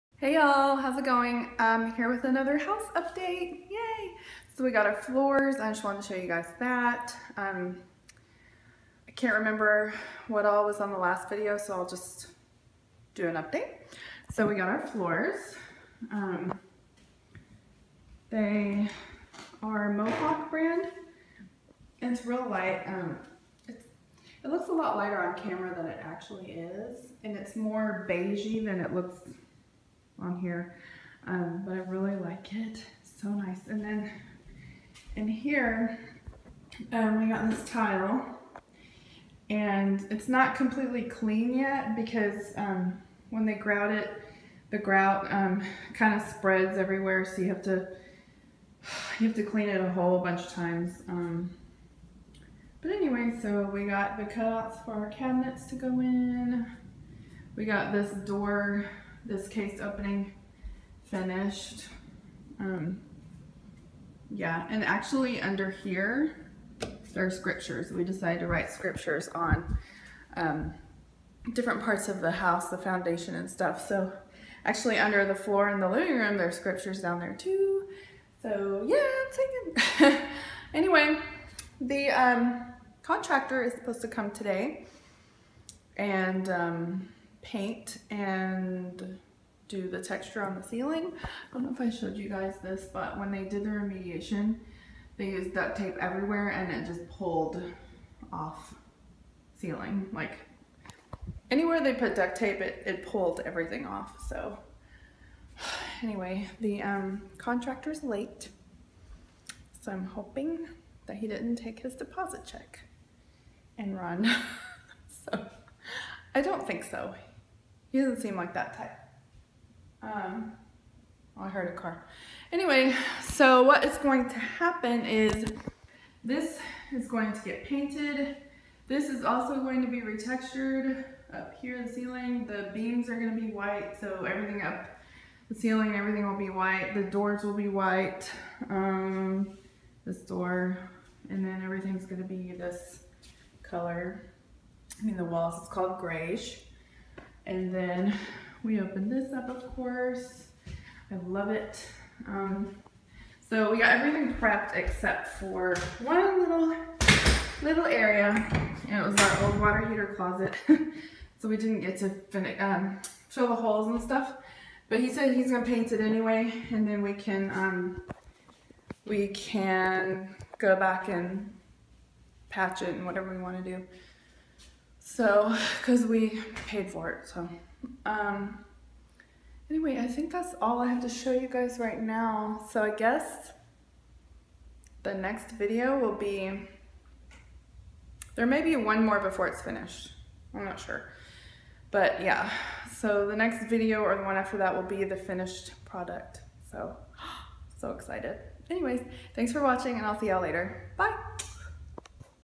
🏠HOUSE REMODEL-Major Steps!
We are in the final major steps of the remodel! Woo!